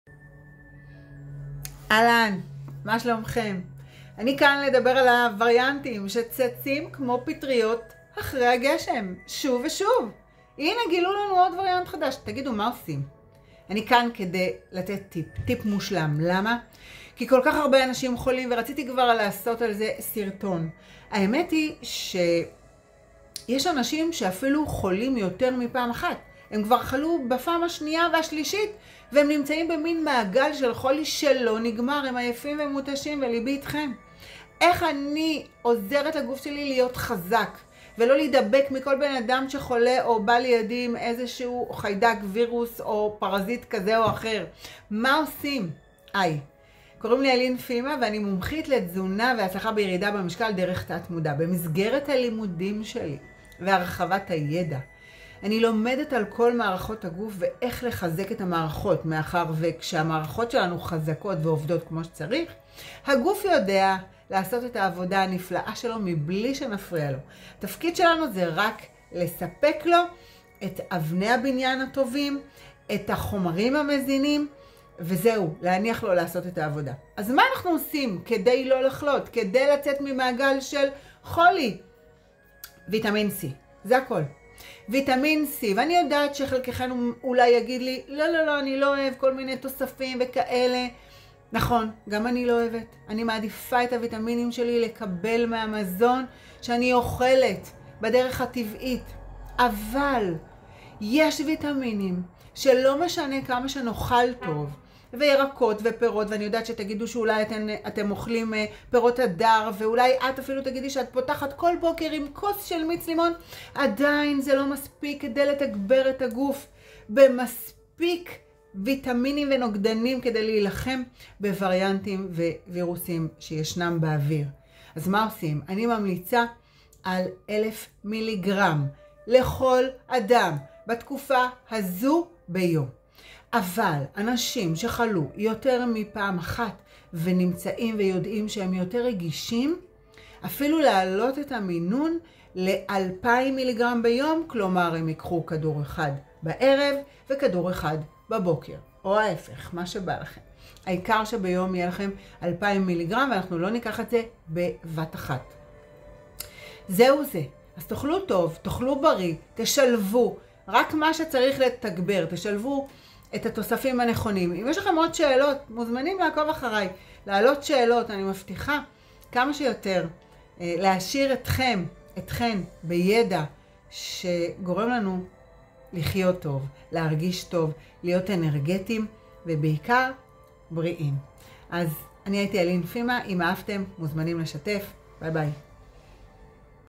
איך לחזק את המערכת החיסונית ולנצח את הקורונה - אלין פימה - שיטת חמשת הכוכבים
תגידו, מה קורה פה?!
כל אדם שלישי חולה או מאומת לקורונה!
ואפילו יש כאלה שחולים יותר מפעם אחת בווריאנטים שונים.
ולכן, החלטתי לצלם לכם סרטון!
דרך נפלאה, מהירה וקלה למניעת חולי
או יציאה ממצב של שפעת
תוך כדי הקלה של הסימפטומים הלא נעימים שמגיעים עם השפעת.

אז מהו הפתרון שאני ממליצה עליו?
ויטמין C, או כמו שאני אוהבת לקרוא לו - ויטמין שיא(!!!)
אני יודעת שחלקכם פחות אוהב תוספי תזונה
גם אני באופן אישי מעדיפה שלא לצרוך תוספים כאלה
היות ולפי אמונתי הגוף יודע לקבל ויטמינים ולספוג אותם טוב יותר
כאשר זה מגיע ממזון ולא מקפסולות ואבקות למיניהן.

אבל ישנם כמה ויטמינים שחשובים לגופנו ולא תמיד אפשר לקבל אותם
מהאוכל שלנו ובטח לא בכמות ובמידה הרצויה.
וזהו למשל הוויטמין C.
ויטמין C הוא אחד מהוויטמינים החשובים בגוף
והוא יכול לעזור לנו במניעת מחלות
על ידי חיזוק של מערכת החיסון.

הוא חיוני ליותר מ2500 תהליכים בגופנו וזה מטורף!!!
למשל: הוא חיוני ליצירת קולגן, מסייע בריפוי פצעים
משפר את ספיגת הברזל.
הוא תורם להאטת תהליך ההזדקנות, נלחם ברדיקלים חופשיים, וירוסים,
חיידקים פתוגניים או פרזיטים למיניהם שמייצרים מחלות בגופנו.
כאשר יש לנו רמה טובה של וויטמין C זה עוזר לגוף שלנו
לקצר את זמן השפעת ולהקל משמעותית על התסמינים,
אך חשוב מכך, יש לו את היכולת למנוע חולי מלכתחילה - ולשם אנחנו מכוונים!

באילו מזונות נמצא וויטמין C:
ירקות ירוקים, פלפל צהוב, פטרוזיליה
פירות הדר, כרובית, מלון, תות שדה
ועוד...

אך מה שחשוב לדעת זה שהגוף לעולם לא יכול
לקבל את הכמות הנכונה על מנת להגן על עצמו בצורה מיטבית
בסרטון אני ממליצה על מינונים שכדאי לקחת מהוויטמין הזה.
ואציין פה שכאשר אתם רוכשים וויטמין C תוודאו שהוא בטכנולוגיה ליפוזומלית
על מנת לשפר את הספיגה בגוף.

המשך חיים נפלאים
והעיקר הבריאות!

אלין פימה
מומחית לתזונה והצלחה בירידה במשקל
דרך תת המודע ובשיטת חמשת הכוכבים
ליווי אישי, סדנאות, קורסים והרצאות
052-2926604